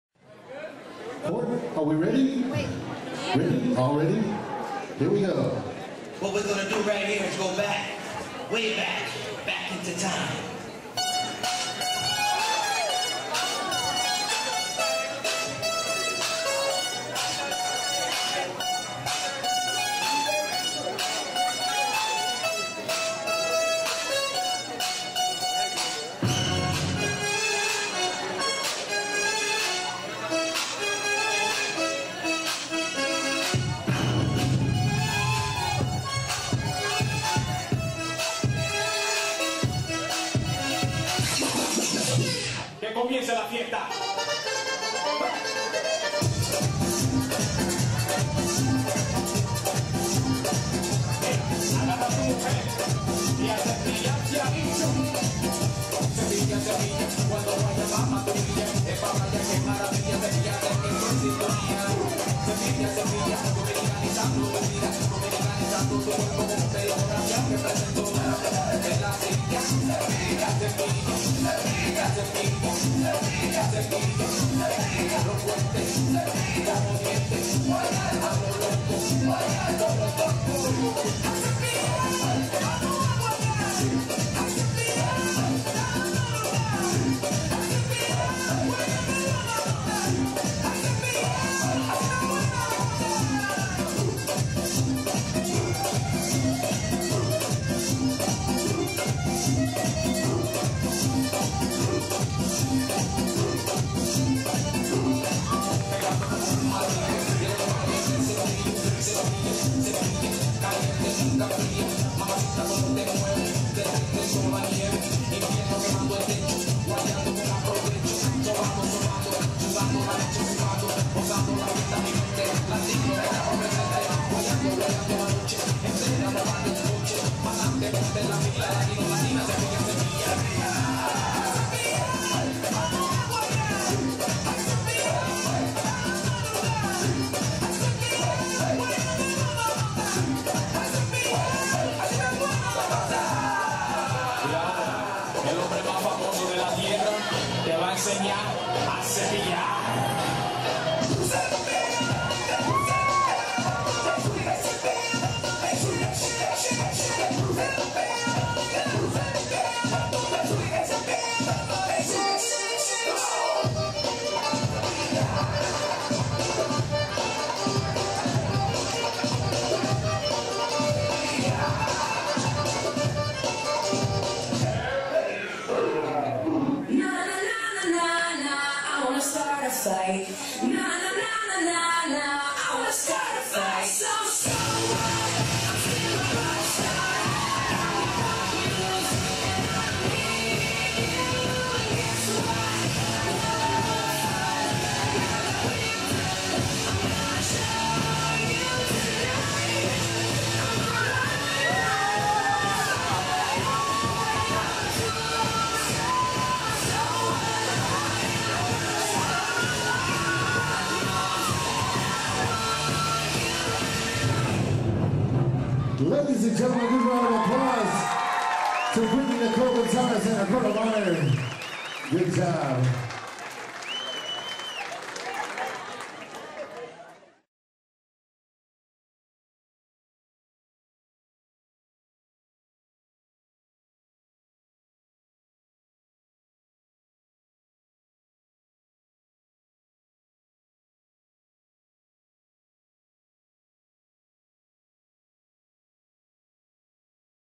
Brittanys Court Dance 2009
Brittany and her court of honor perform a special choreographed dance at her Quinceanera.